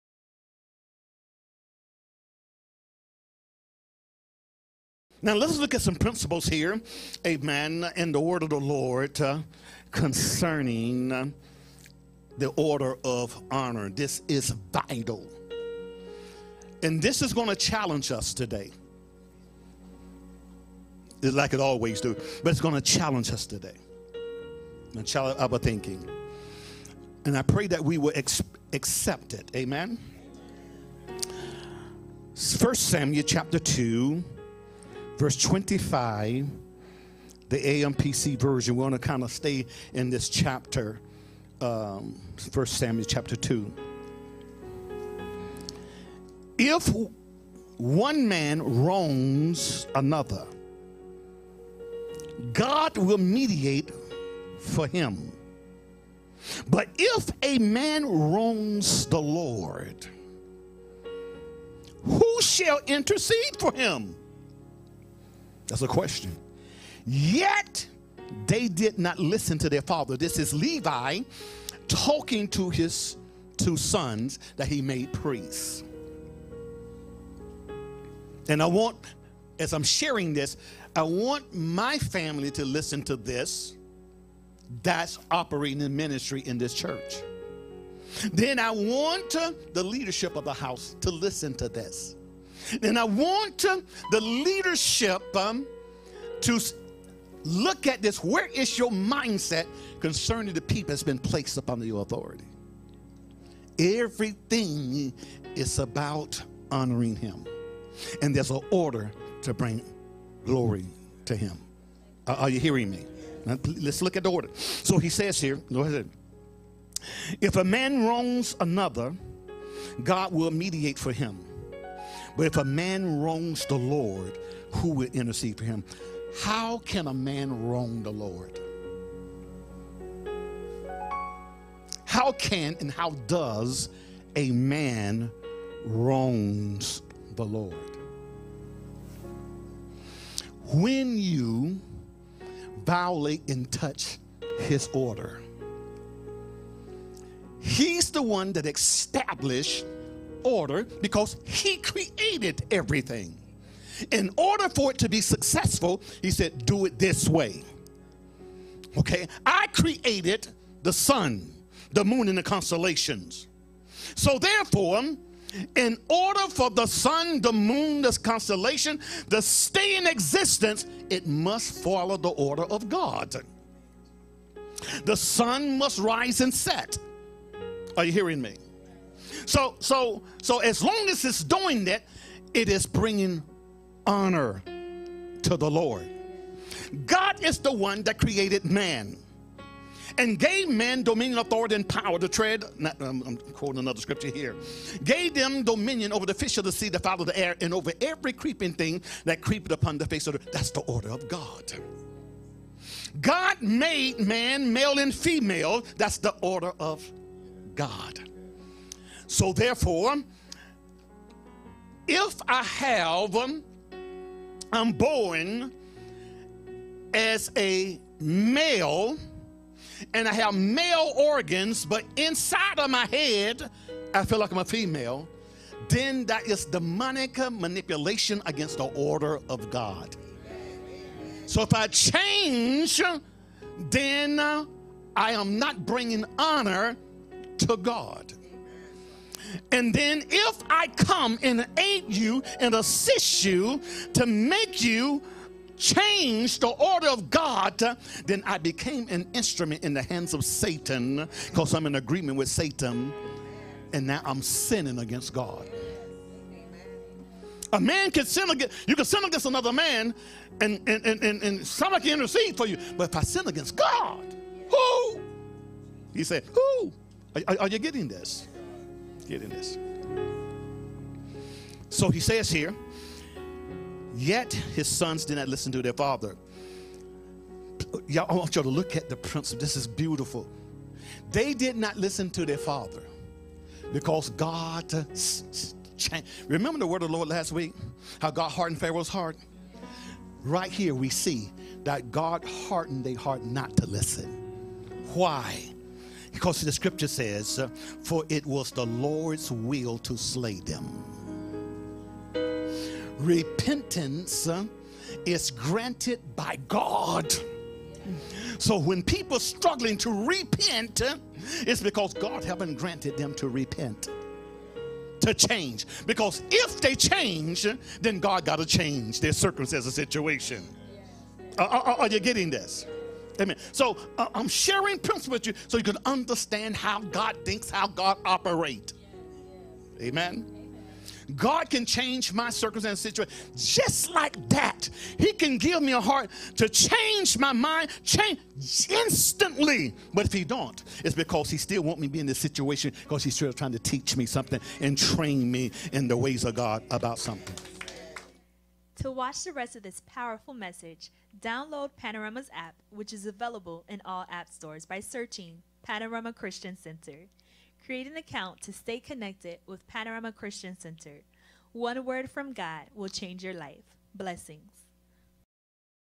The Order of Honor Part II | Apostle Emanu-el J. McKenzie TH.D
Text PCCMIAMI to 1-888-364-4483 to give to Panorama or to Emanu-el J. McKenzie Ministries Inc.!
In Luke 17:11-19; “there were ten lepers who went to Jesus to be healed.” Jesus gave them instructions. When they followed the instructions, they were healed. One leper returned and glorified God and then thanked Jesus. Notice the Order of Honor; the leper gave glory to the source of his healing; then he thanked Jesus; the instrument of his healing. You cannot separate the source from the instrument of honor. If we dishonor the instrument; we dishonor the source. Have you returned to give thanks to the instrument of honor for allowing the source of honor to give what you need through them?

Blessings